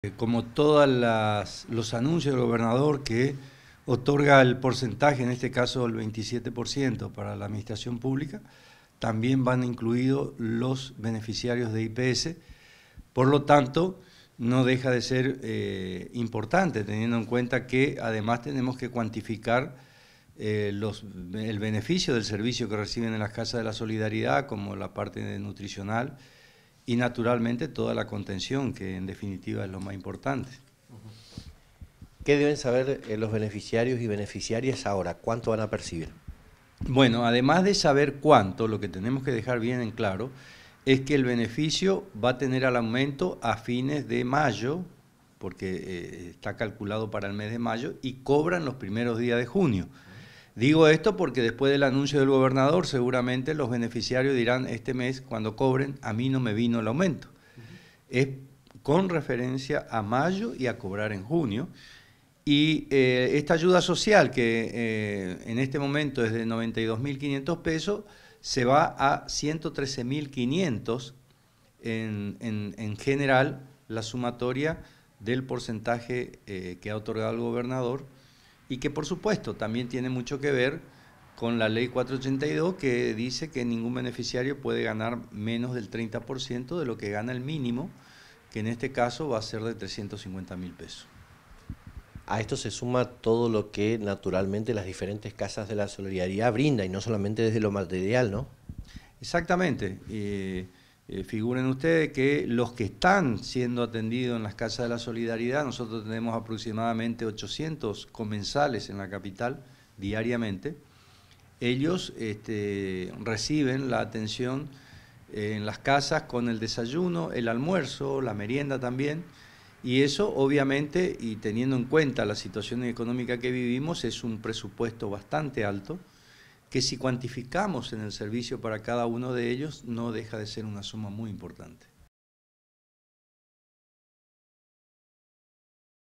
BENEFICIARIOS DEL IPS RECIBIRÁN EL 27% DE AUMENTO ANUNCIADO POR INSFRÁN
LOS BENEFICIARIOS DEL INSTITUTO DE PENSIONES SOCIALES DE FORMOSA TAMBIÉN RECIBIRÁN EL AUMENTO DEL 27% ANUNCIADO POR EL GOBERNADOR GILDO INSFRÁN.
LA AYUDA SOCIAL A BENEFICIARIOS DEL IPS QUE EN LA ACTUALIDAD ES DE 92.500 PESOS, PASARÁ A 113.500.